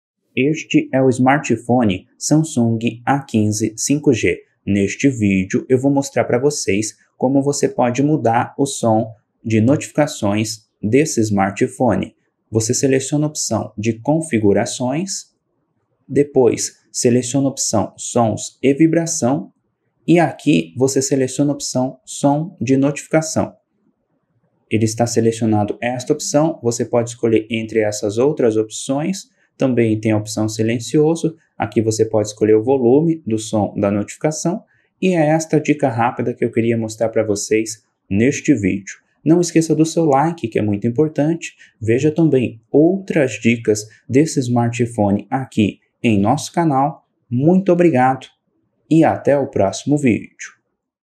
Como mudar o som de notificação no Samsung A15 5G
Neste vídeo vou te ensinar como mudar o som de notificação no Samsung A15 5G